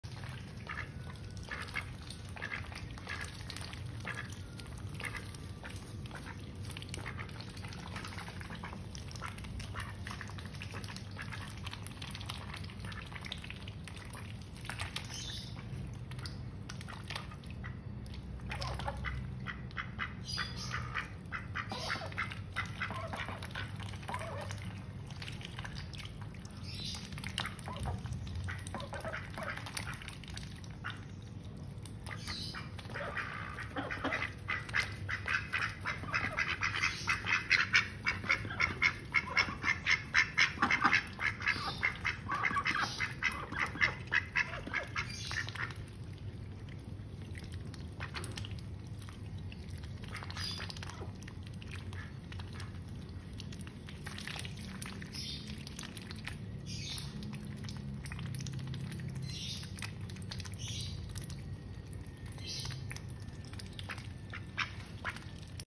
Ducks on 26 February 2023
This weekend our ducks enjoy raining day at the backyards.